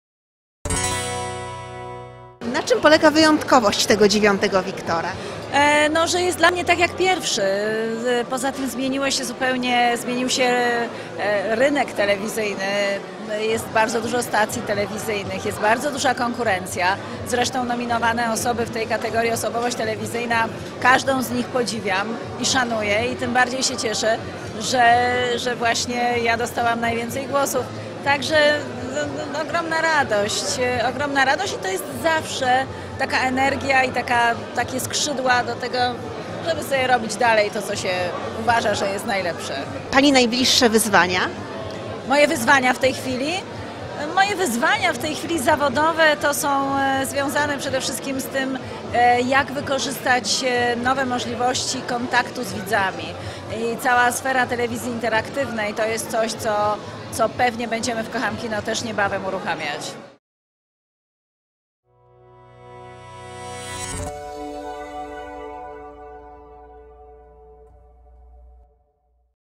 Torbicka: Dziewiąty Wiktor tak jak pierwszy
Zobacz więcej na | Grażyna Torbicka otrzymała Wiktora w kategorii osobowość roku. Jak podkreśliła, nagroda jest dla niej ważna, bo w telewizji pojawiła się duża konkurencja